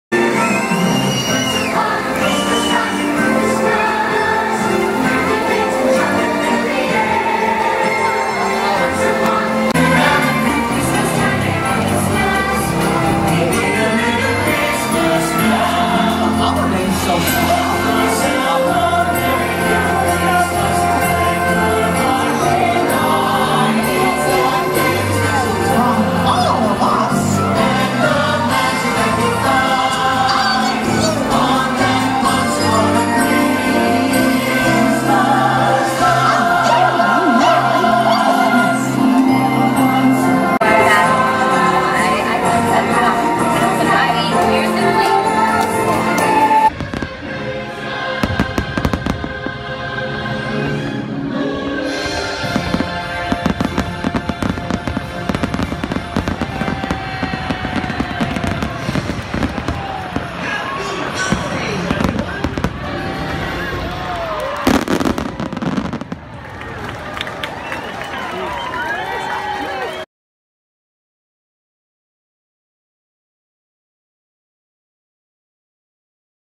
NAVIDAD EN MAGIC KINGDOM, DISNEY 2017
Un vistazo a la Navidad del parque Magic Kingdom en Walt Disney World, donde protagonizan Elsa y Anna de la película Frozen, Mickey Mouse y el Castillo de Cinderella (Cenicienta), que se convierte en un palacio helado.